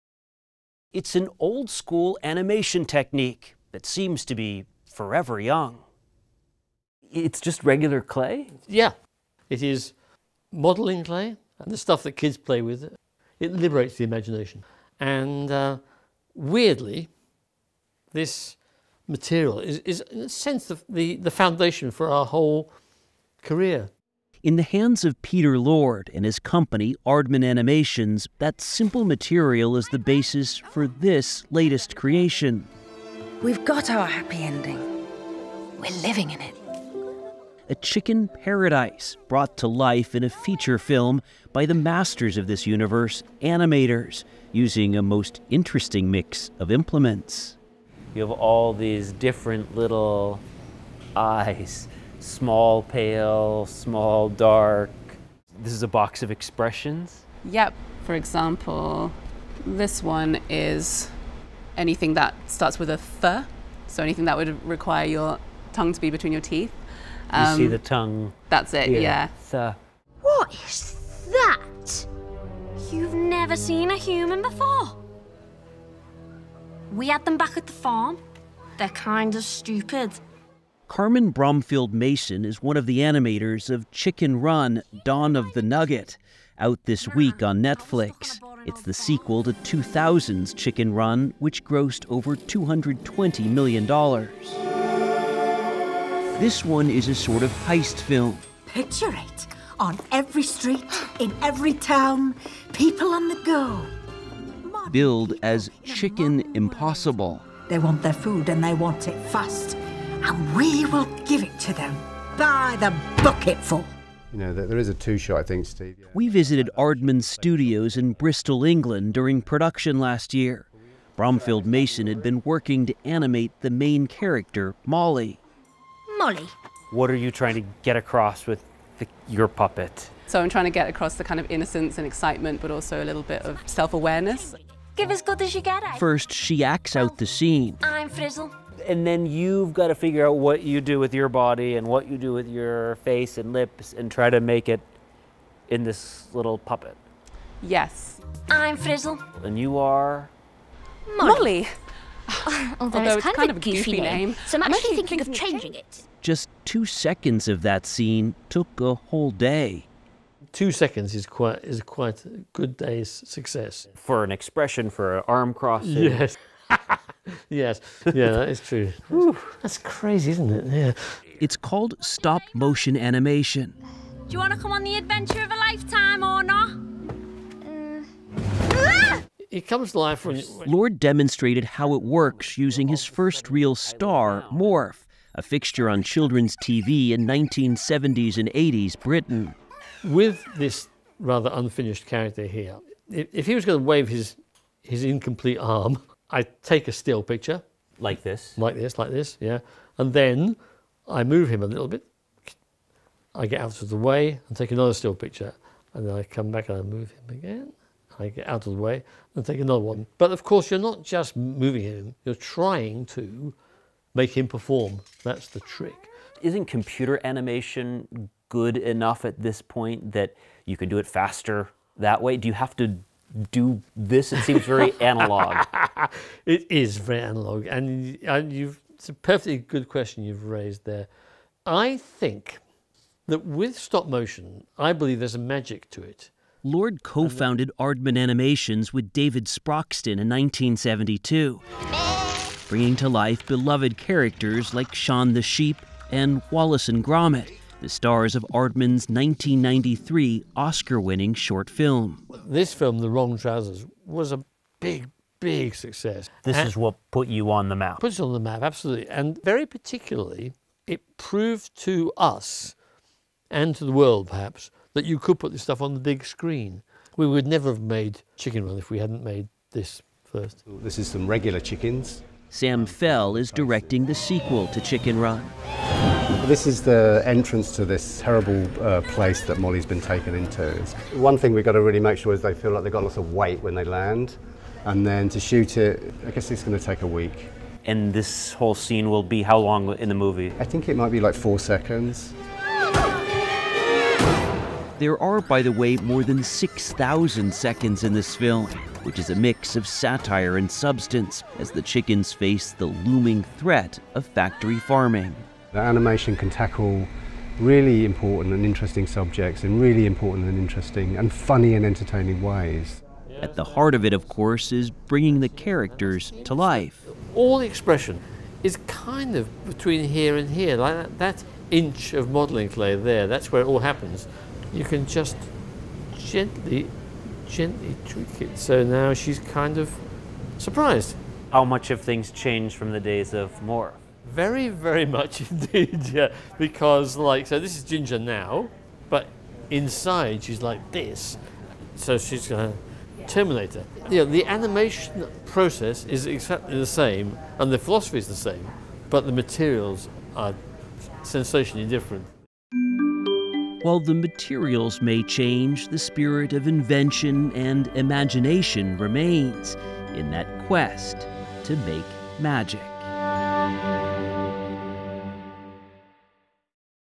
Aardman Animations: Creating the magic of stop-motion
The animation wizards behind Wallace & Gromit and Shaun the Sheep are back with a sequel to their 2000 hit feature, "Chicken Run." Correspondent Seth Doane visits the Bristol, England studios of Aardman Animations, where artists have painstakingly filmed "Chicken Run: Dawn of the Nugget," a comic adventure in which daredevil chickens seek to rescue their precious hen from a dastardly factory farm. (Think "Chicken: Impossible.")